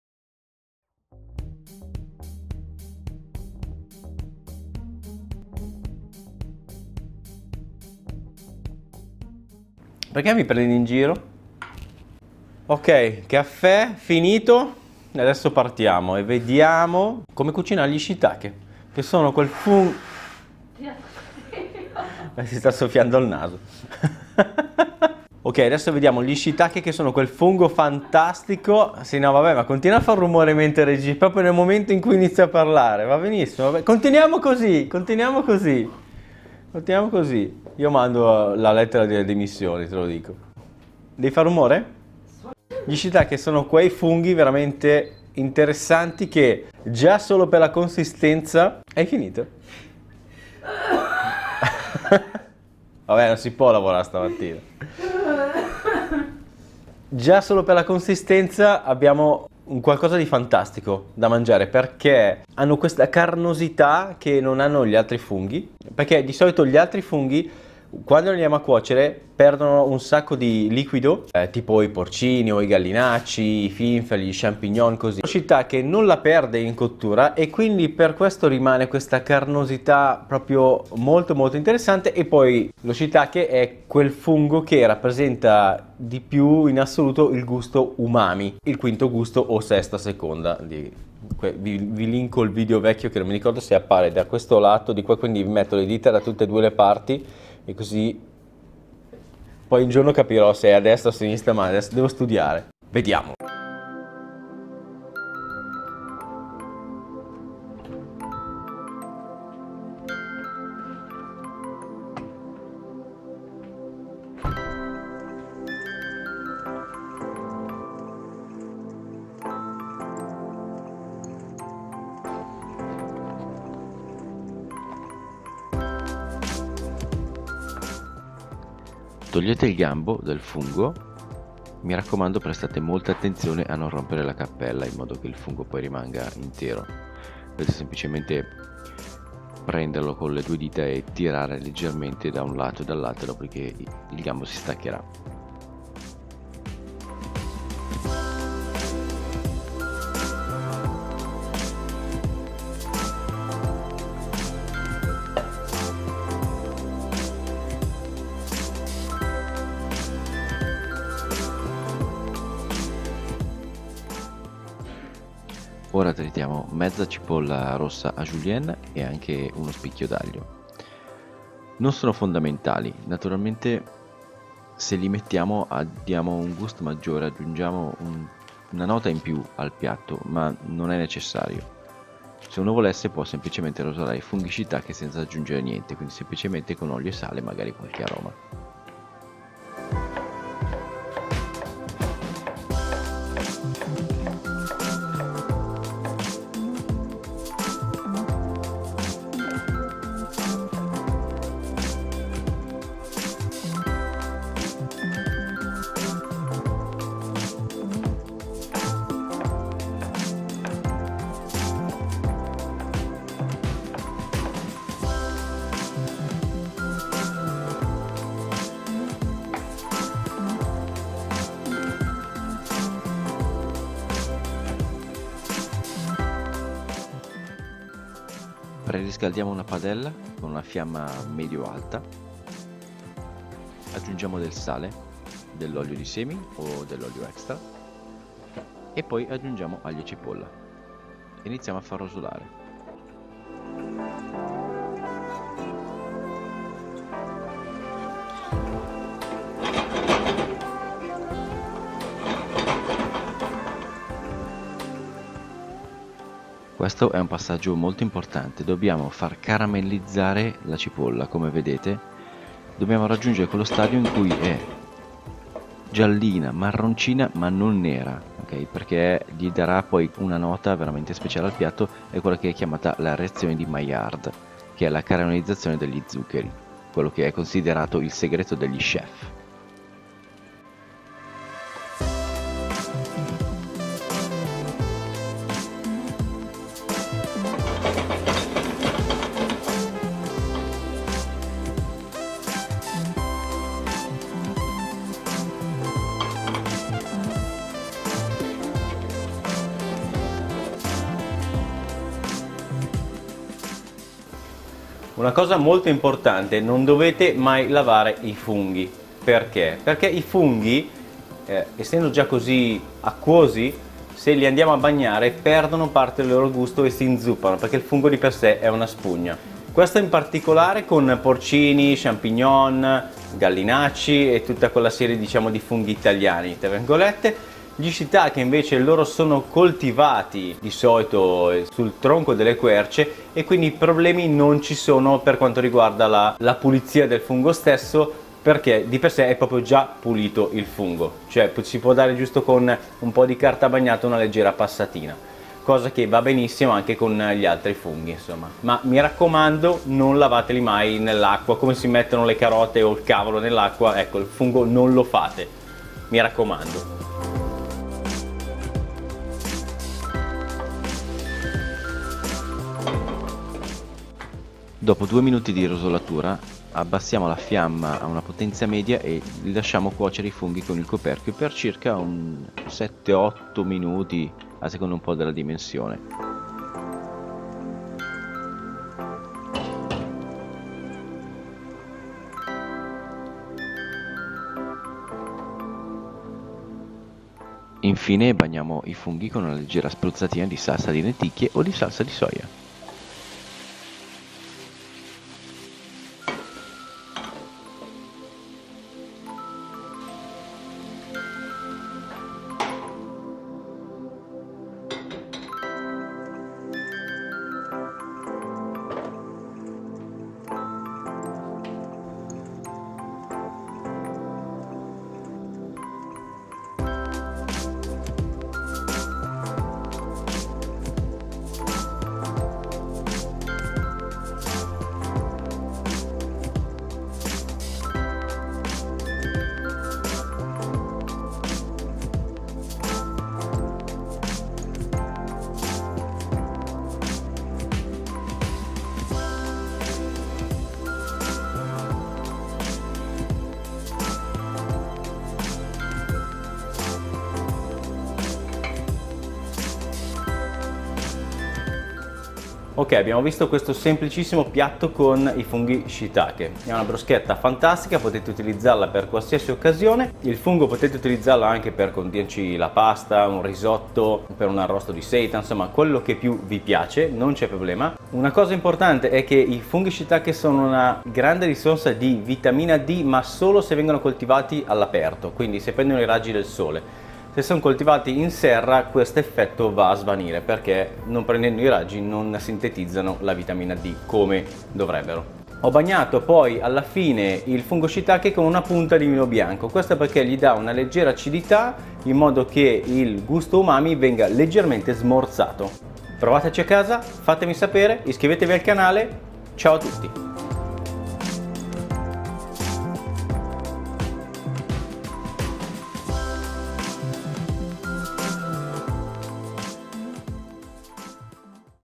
Come cucinare i funghi Shiitake 🥢🔪 alla Chef Davide 👨‍🍳
ATTENZIONE: la cipolla è tagliata a brunoise, non a julienne come dico nel video.
Perdonatemi. Purtroppo registro gli audio separatamente dalle riprese alle 7 circa del mattino... e per il momento è ancora tutti lavoro che faccio gratuitamente per voi :D

Dedichiamo la video ricetta di oggi ai Funghi Shiitake biologici coltivati in Italia.
Sono i funghi preferiti da Chef Davide Maffioli, soprattutto perché sanno regalare un gusto Umami unico nel regno funghi.
Li mangiate?
Avete mai provato a cucinarli?
Vediamo insieme come renderli al meglio!

o Vuoi postare la foto delle ricette che hai provato?